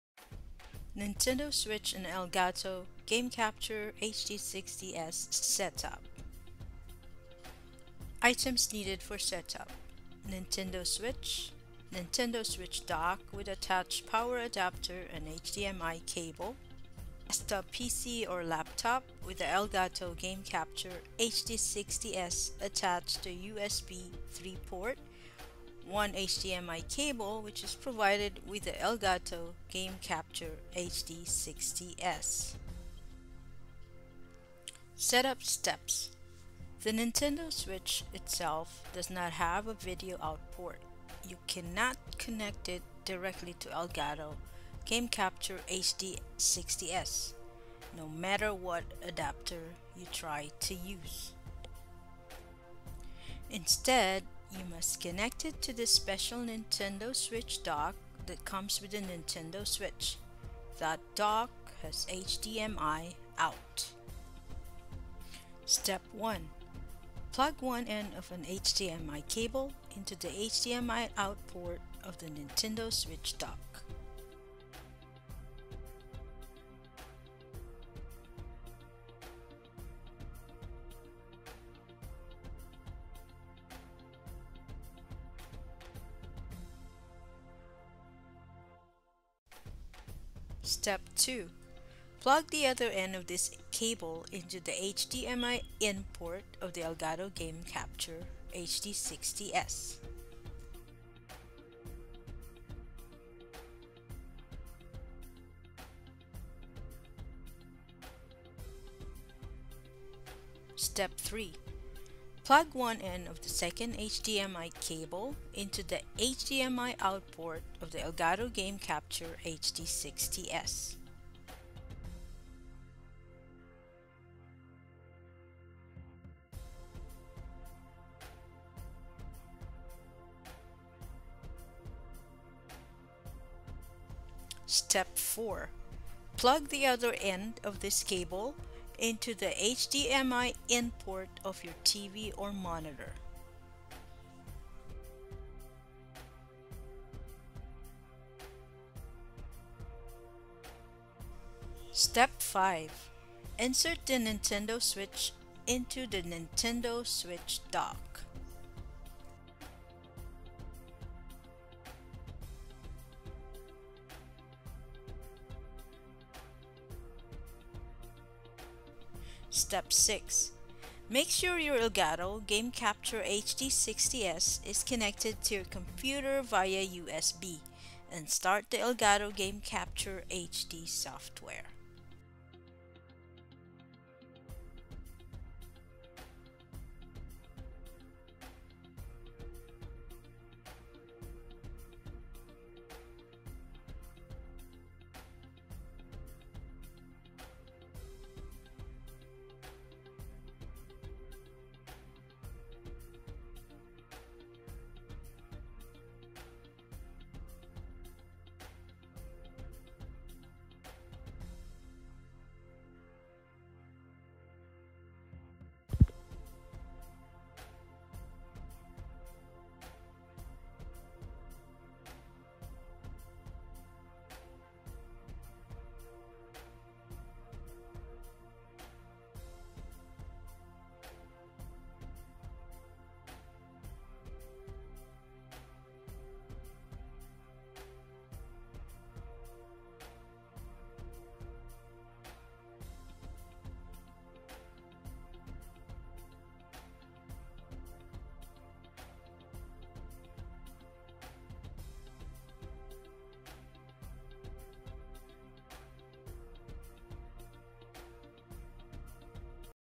How to connect your Nintendo Switch to your Elgato HD60 S - Nintendo Switch and Elgato HD60 S Set Up
Items Needed for Setup

• Nintendo Switch

• Nintendo Switch Dock with attached power adapter and HDMI cable

• Desktop PC with the Elgato Game Capture HD60 S attached to an USB 3.x port.

• 1 HDMI cable, which is provided with the Elgato Game Capture HD60 S.
Setup Steps

The Nintendo Switch itself does not have a video out port - you cannot connect it directly to Elgato Game Capture HD60 S, no matter what adapter you try to use.

Instead, you must connect it to the special Nintendo Switch Dock that comes with the Nintendo Switch.  That Dock has HDMI out.

1) Plug one end of an HDMI Cable into the HDMI OUT port of the Nintendo Switch Dock.

2) Plug the other end of this cable into the HDMI IN port of the Elgato Game Capture HD60 S.

3) Plug one end of the second HDMI Cable into the HDMI OUT port of the Elgato Game Capture HD60 S.

4) Plug the other end of this cable and plug it into an HDMI IN port of your TV/Monitor.

5) Insert the Nintendo Switch into the Nintendo Switch Dock.

6) Make sure your Elgato Game Capture HD60 S is connected to your computer via USB, and start the Elgato Game Capture HD software.